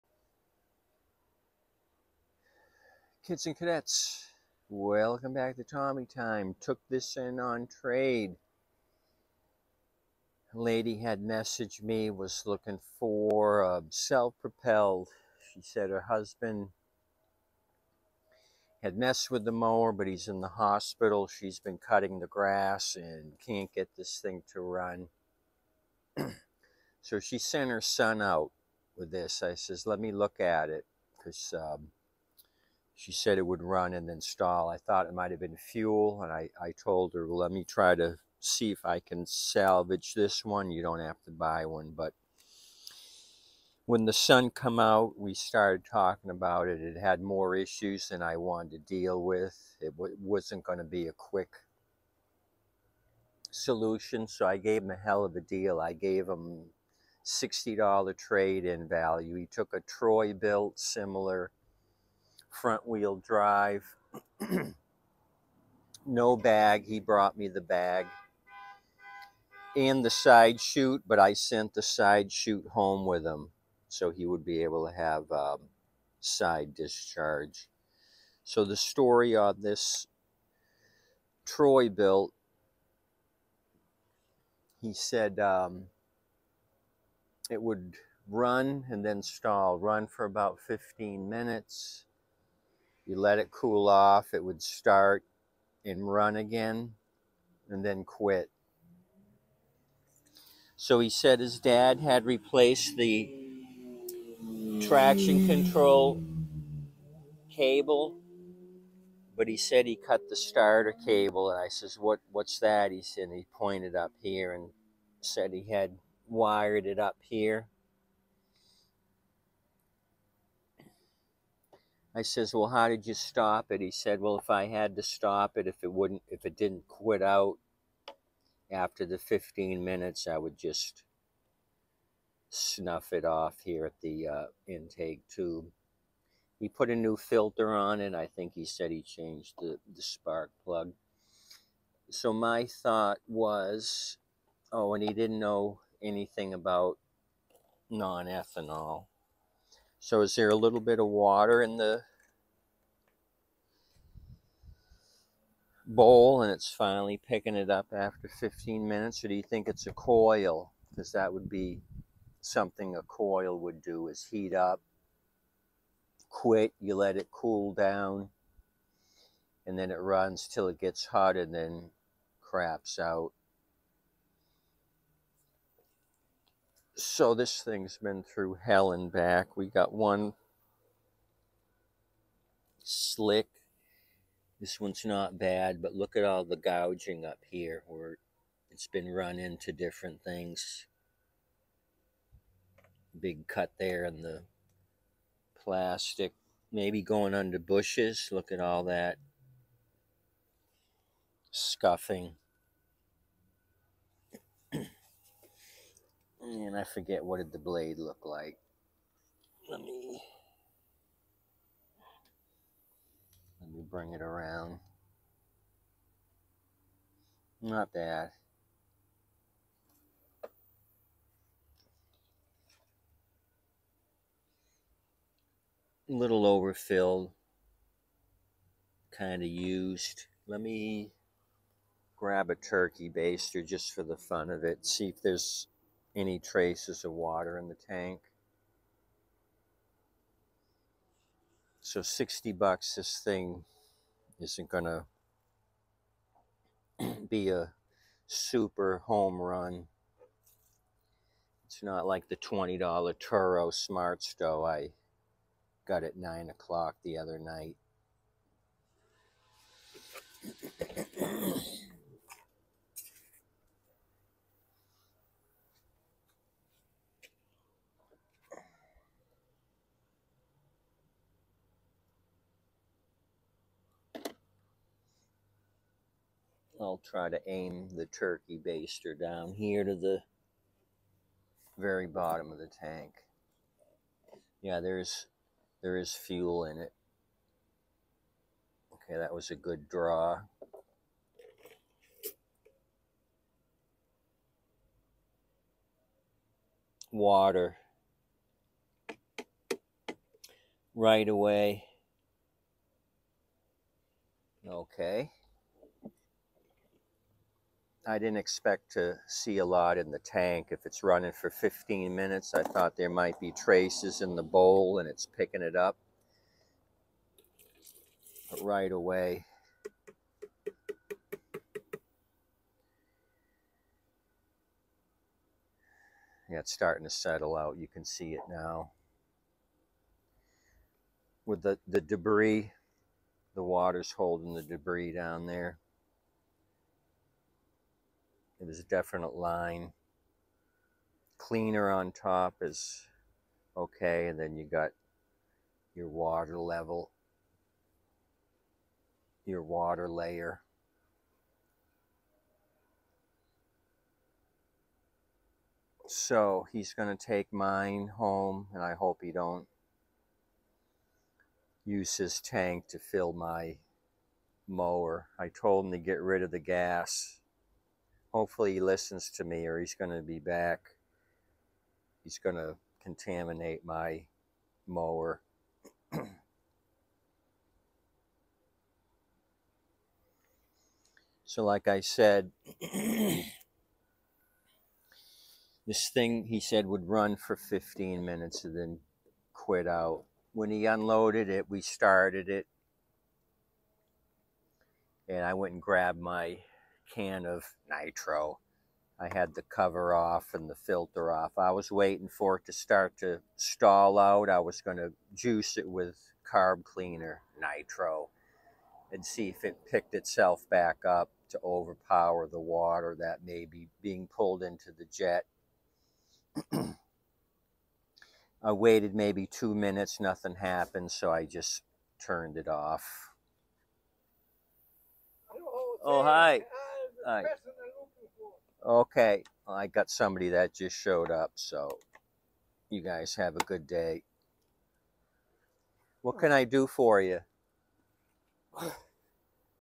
Troy-Bilt TB200 Self Propelled Lawn Mower. I gave $60 Trade in Value for this Giant Turd.!!
Not the best of deals but, i will make it run no matter what.
TB200 with 625EX engine,  FWD.